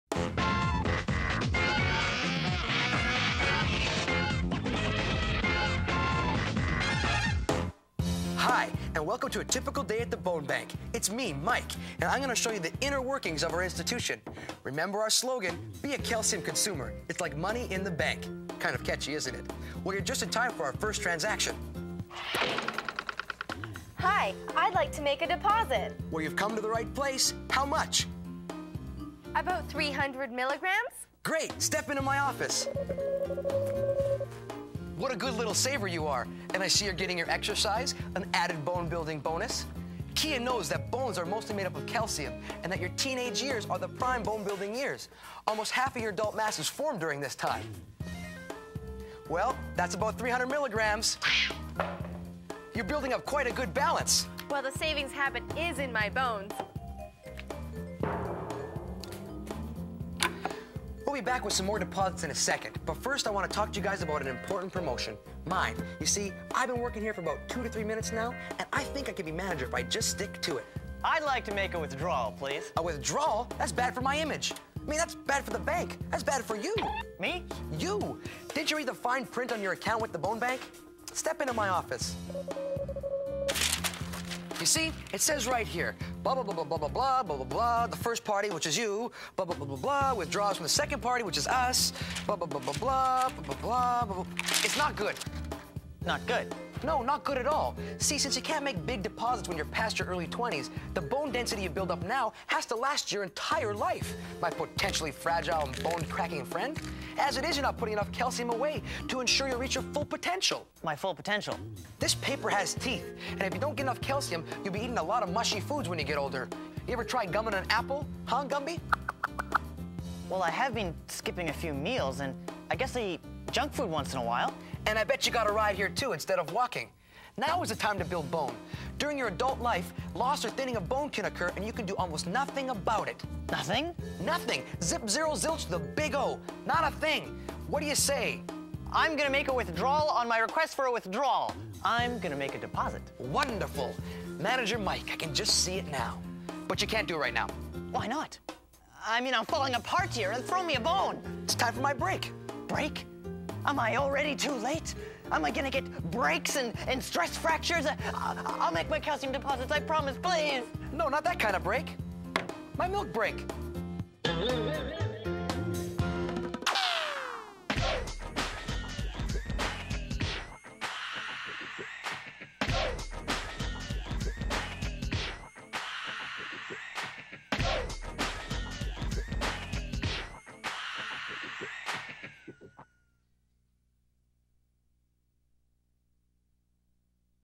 Bone Bank.mp4
How is the balance in your Bone Bank account? The customers at the Bone Bank make "deposits" of calcium to help make sure they have healthy and strong bones.